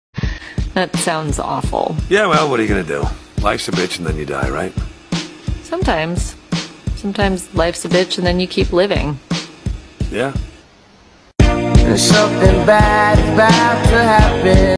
MS is awful but life goes on so now I paint my illness to raise awareness of it 💪
In july of 2022 I was diagnosed with the chronic illness Multiple Sclerosis. It truly beat me down.. but life goes on, now I paint each aspect of MS to raise awareness of this illness to help other MS patients and their loved ones ❤

And yeah, these things don't make you stronger.. they just make you wish you've died.
But whatever.